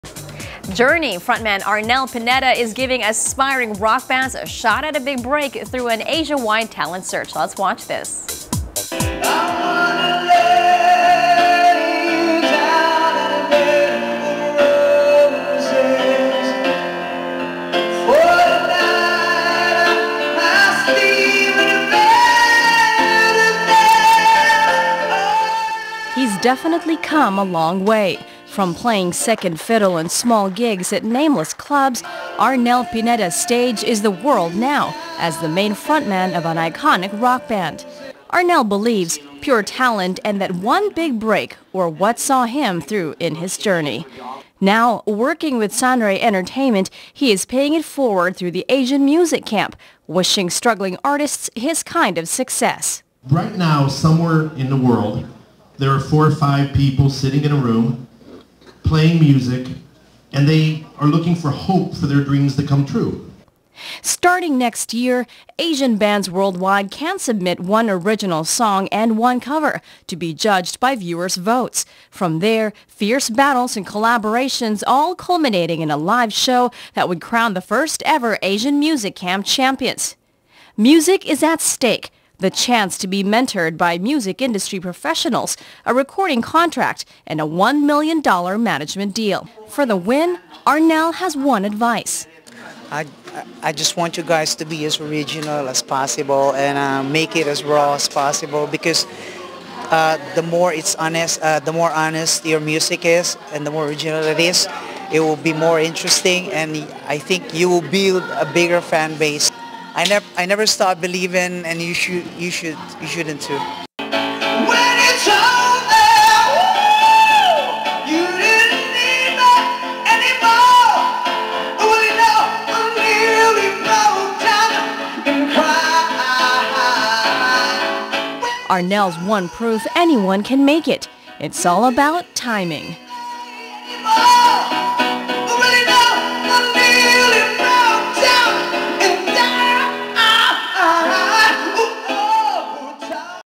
Arnel Pineda's Search For The Next Big Band From Asia
Journey frontman Arnel Pineda is giving aspiring rock bands
a shot at a big break through an Asia-wide talent search.
Let's watch this.